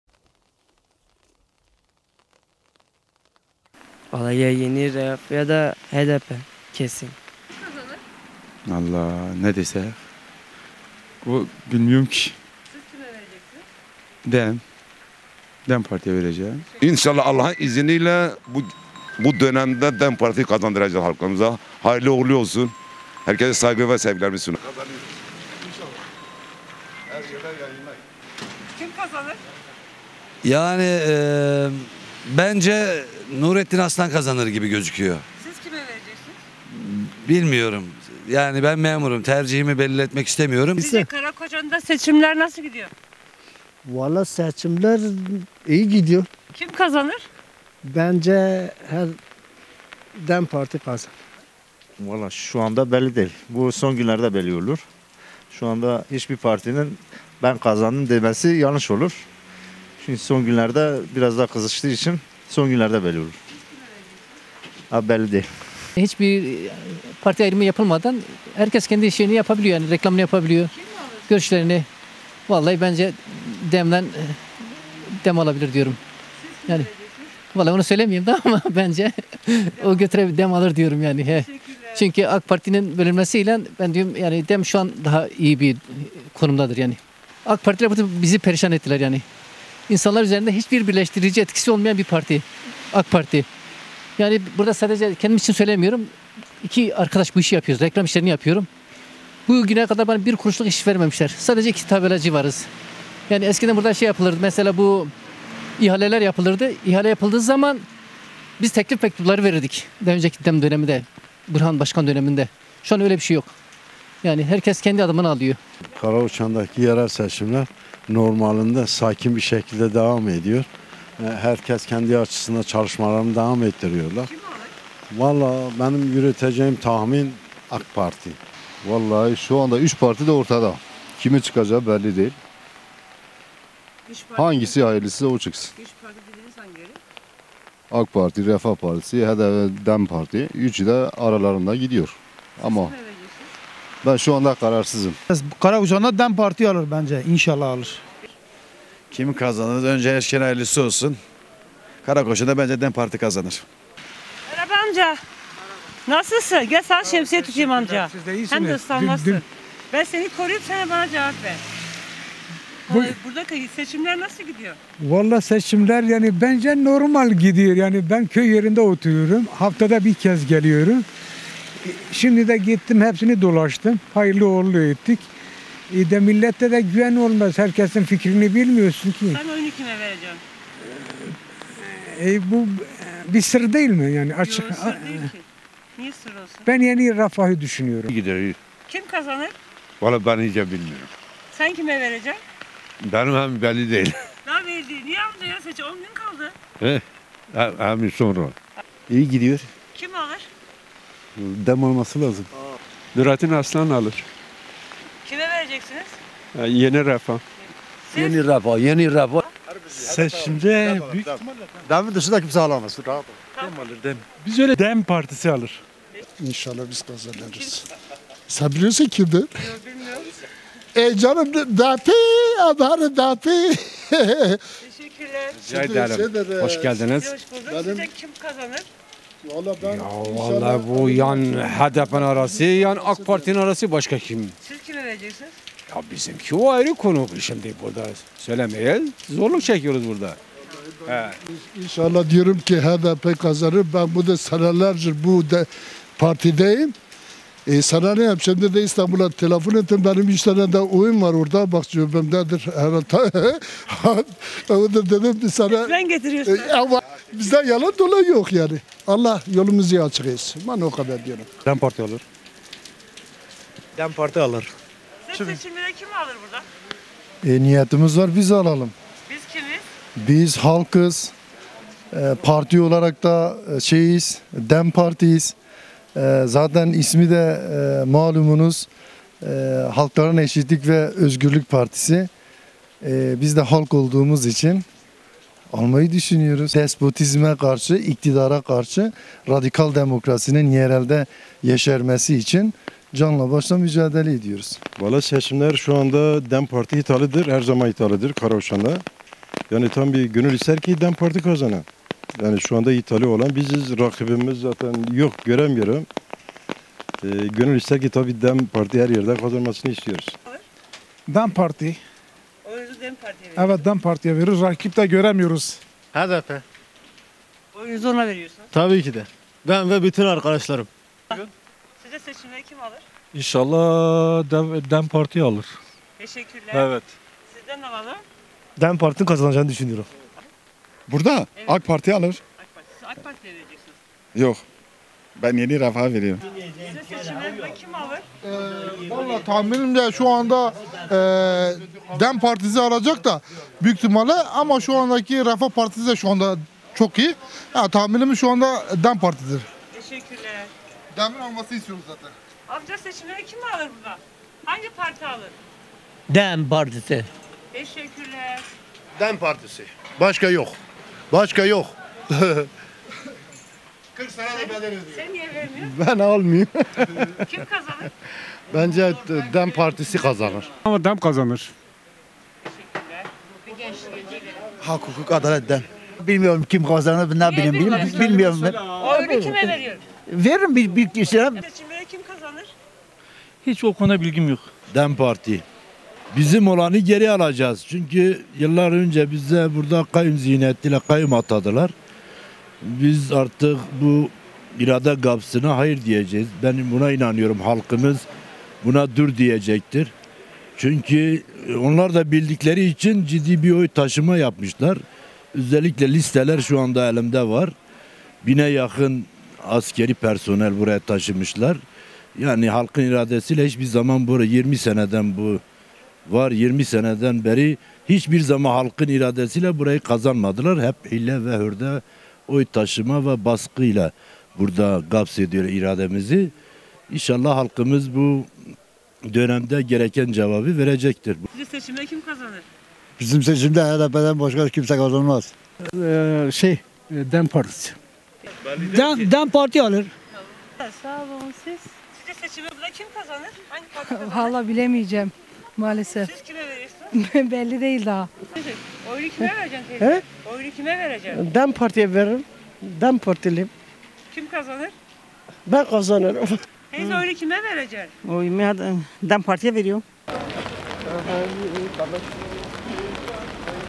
Karakoçan'da sokağın nabzı: DEM Parti kazanabilir
PİRHA- 31 Mart Yerel Seçimlerine sayılı günler kala adaylar arasındaki yarışta tansiyon yükselirken, Elazığ'ın Karakoçan ilçesinde yurttaşlara hangi partiye oy vereceklerini ve seçim sonucuna dair tahminlerini sorduk. AKP'li belediye başkanından memnun olmayan yurttaşların DEM Parti'nin kazanma olasılığının yüksek olduğunu belirttiler.